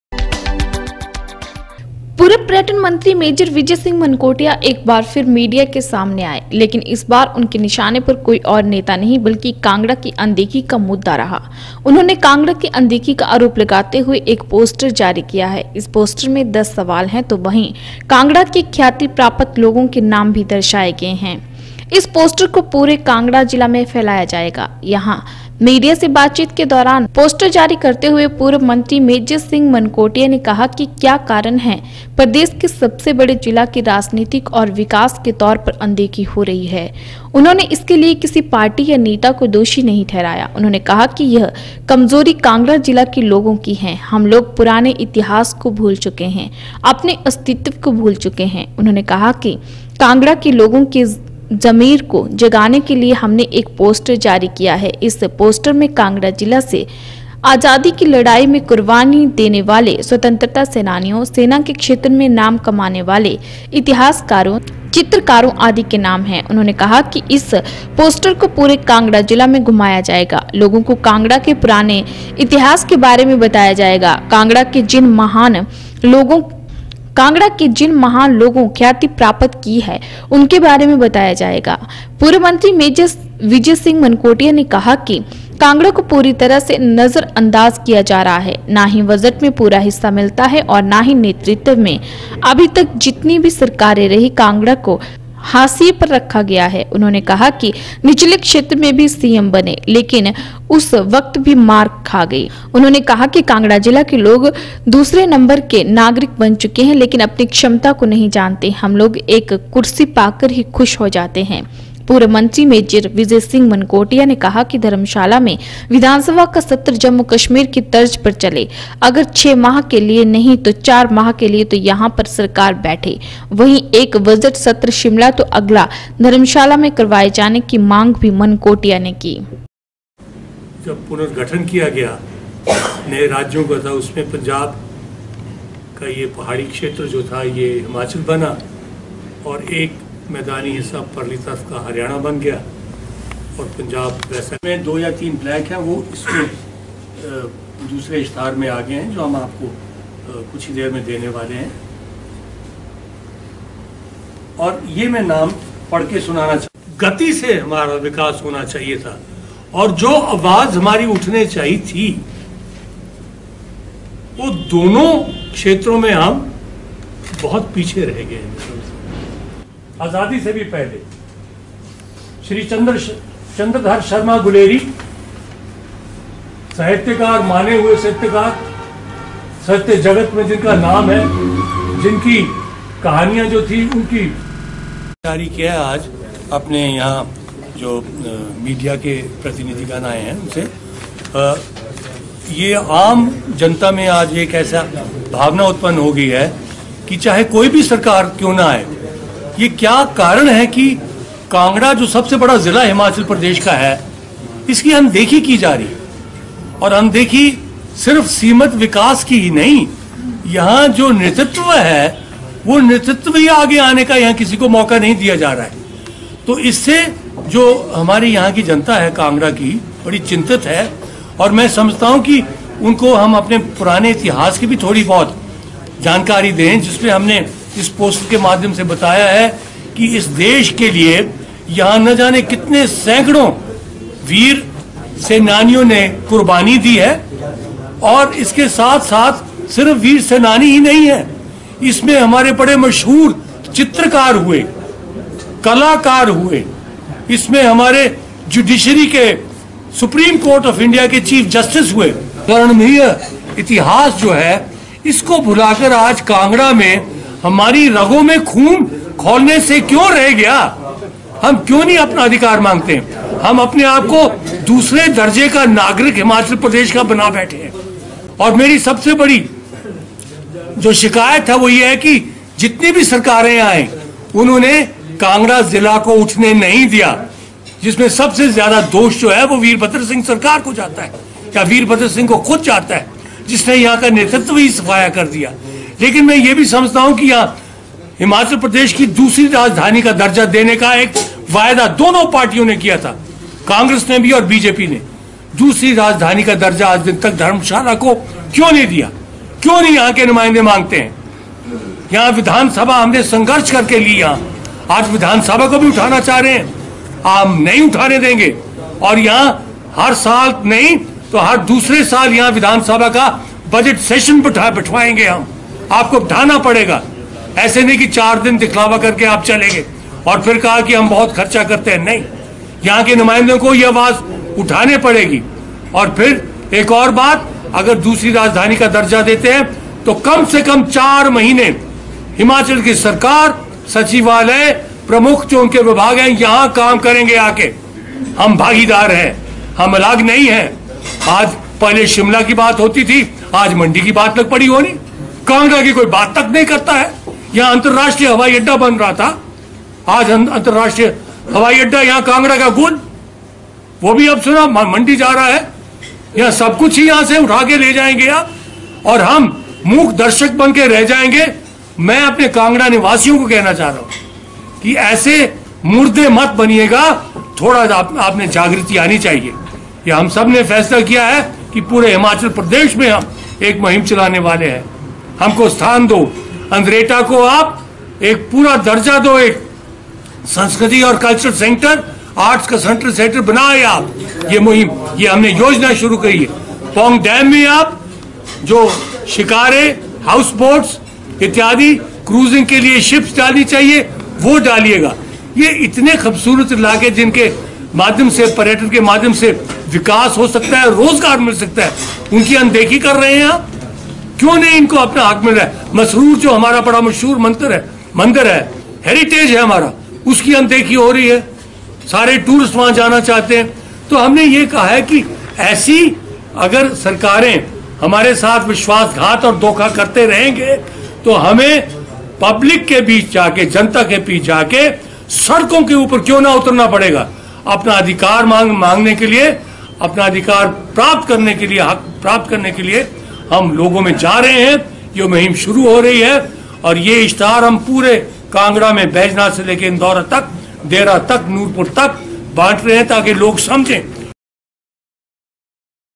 धर्मशाला में मानकोटिया ने जारी किया पोस्टर, कांगड़ा की अनदेखी के लगाए आरोप पूछे 10 सवाल
पूर्व पर्यटन मंत्री मेजर विजय सिंह मानकोटिया एक बार फिर मीडिया के सामने आए। लेकिनए इस बार उनके निशाने पर कोई नेता नहीं बल्कि कांगड़ा की अनदेखी का मुद्दा रहा। उन्होंने कांगड़ा की अनदेखी का आरोप लगाते हुए एक पोस्टर जारी किया है। इस पोस्टर में दस सवाल हैं तो वहीं कांगड़ा के ख्याति प्राप्त लोगों के नाम भी दर्शाए गए हैं। इस पोस्टर को पूरे कांगड़ा जिला में फैलाया जाएगा।